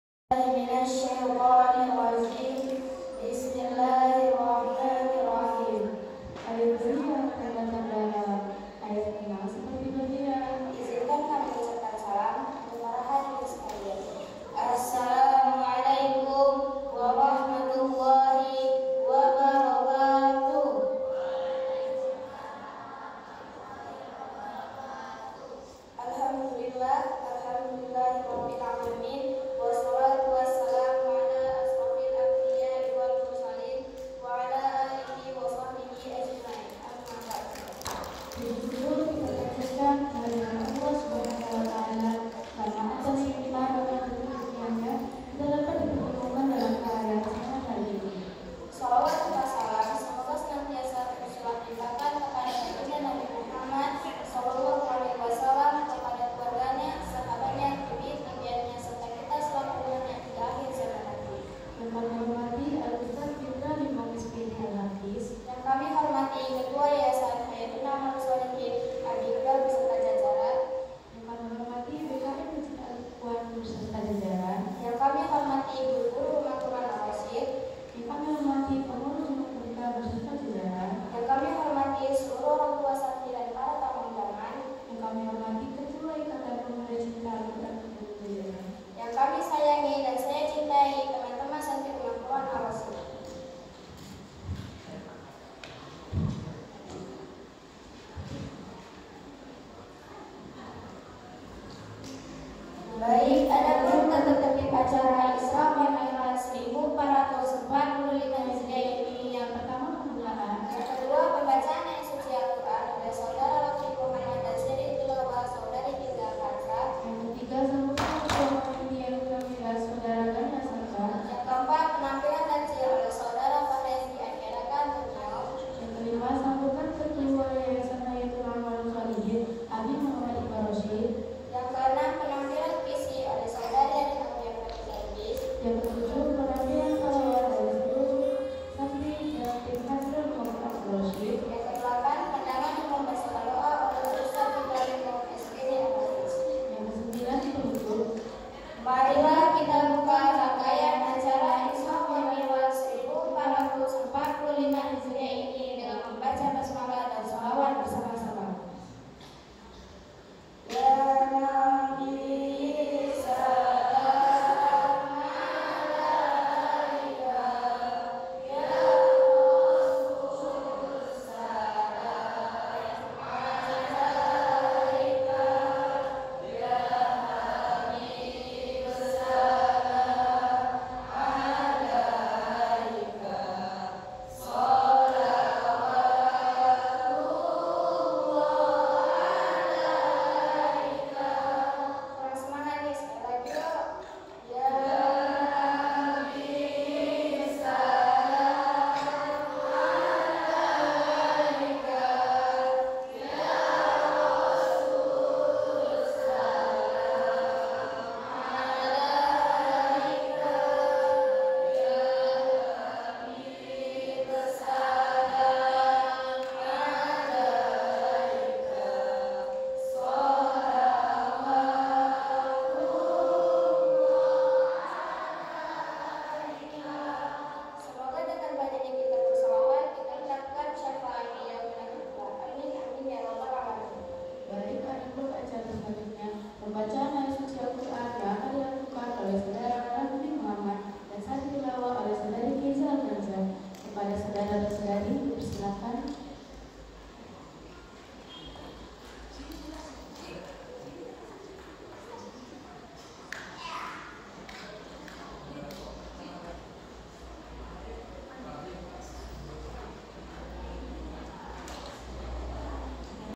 Kak Ataya dan Kak Naira membuka Acara Isra' mi'raj 1445 H (2024)
خَيْرُكُمْ مَنْ تَعَلَّمَ الْقُرْآنَ وَعَلَّمَهُ .

“Sebaik-baik kalian adalah orang yang belajar Al-Qur`an dan mengajarkannya.”

Pastikan :
Tag teman yg gemar tilawah

Semoga bermanfaat 🙏